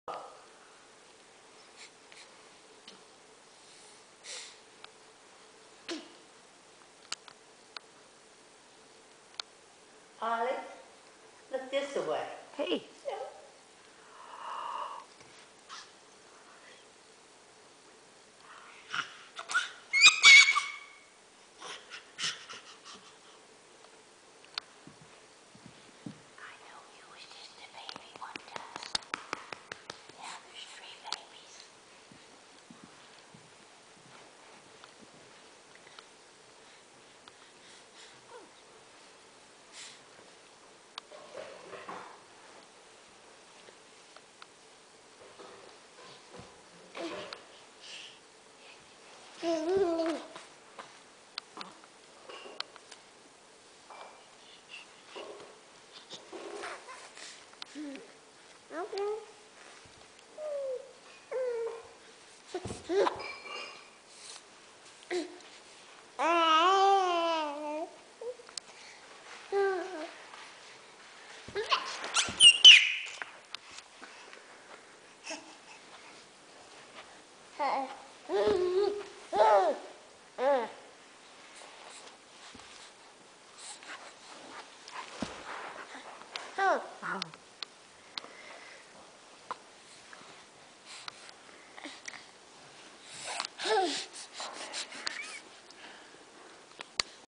Oct 2009 045
Olive and her new doll.  Thank you Cousin Morgan!  We named her Sally Anne.